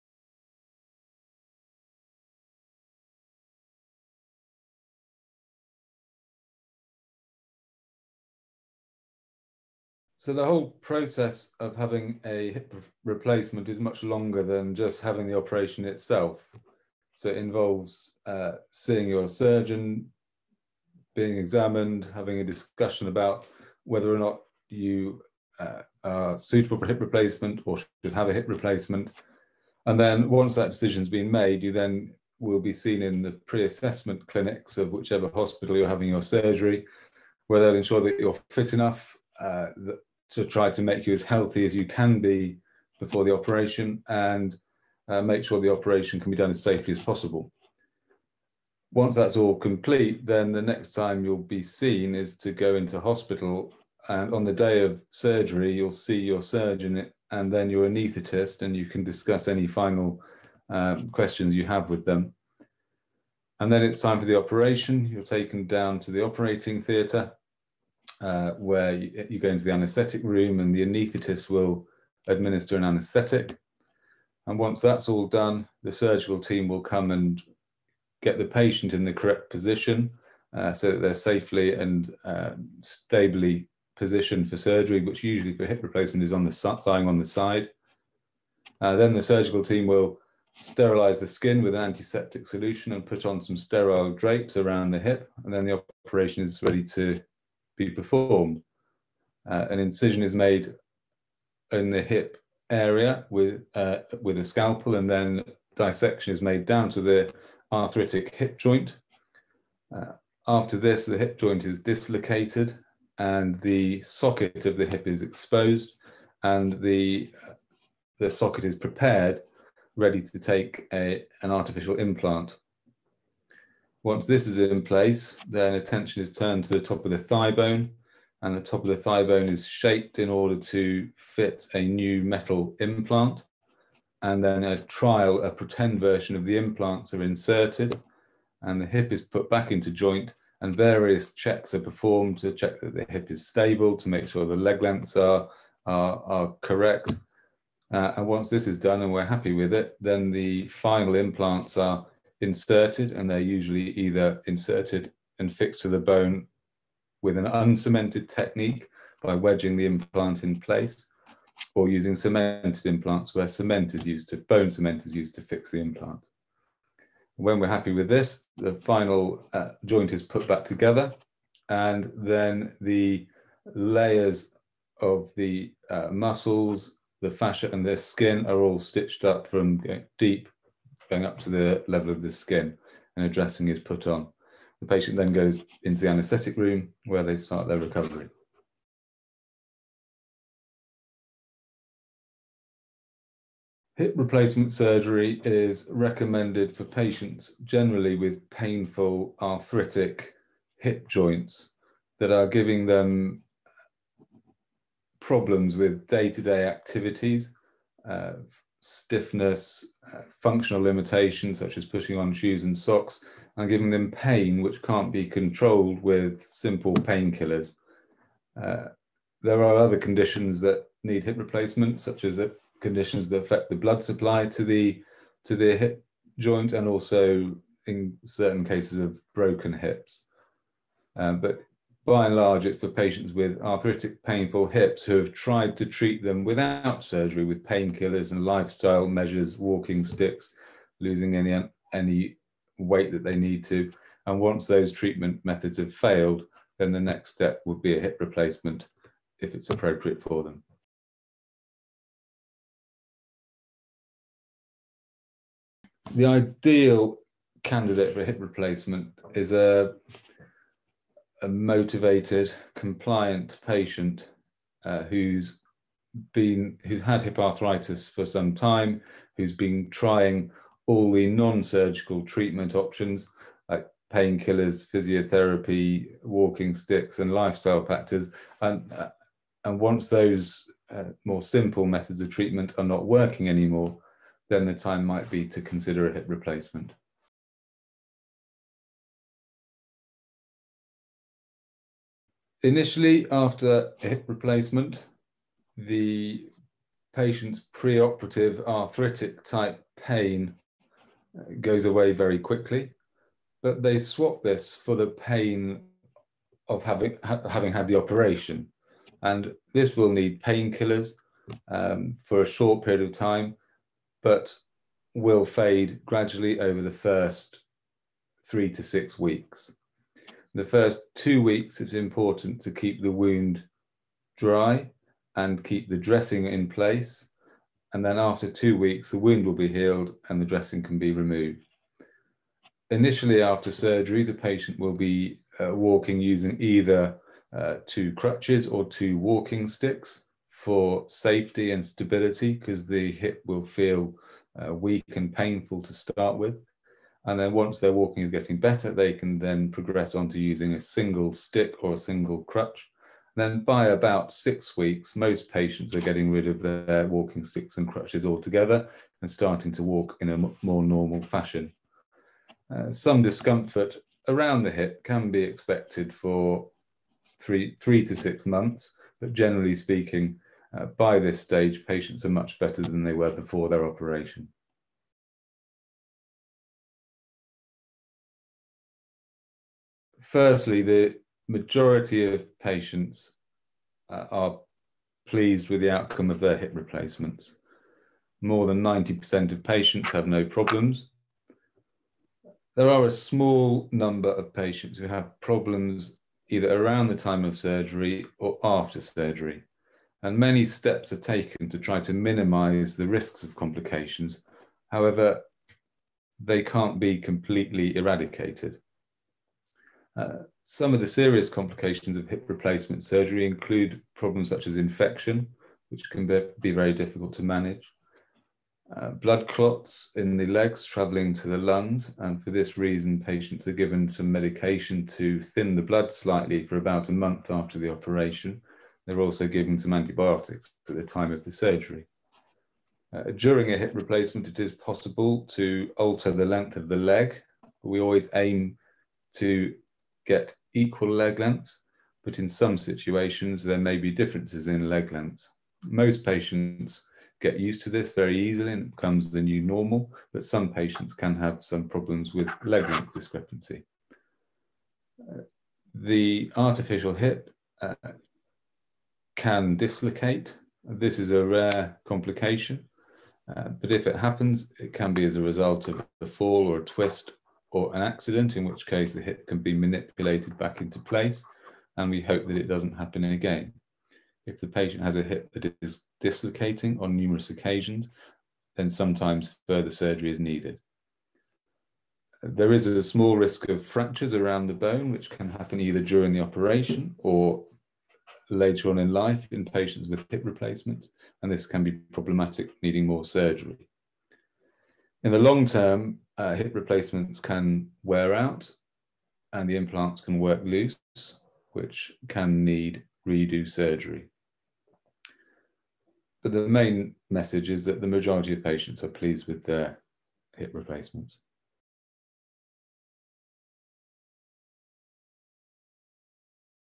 Hip replacement: frequently-asked questions - Online interview
A hip replacement is a surgical procedure to remove and replace a damaged hip joint, usually caused by arthritis, with an artificial implant.

Mr Simon Hoskinson, leading trauma and orthopaedic surgeon, explains what are the different steps involved in a hip replacement, when this surgery is indicated and what are the main potential complications after the procedure.

00:00 How exactly is a hip replacement performed?
03:10 When is this surgery recommended?
04:12 Who is the ideal candidate for a hip replacement?
04:48 What can patients expect after surgery? What is recovery time like?
06:16 What are the main potential post-surgical complications?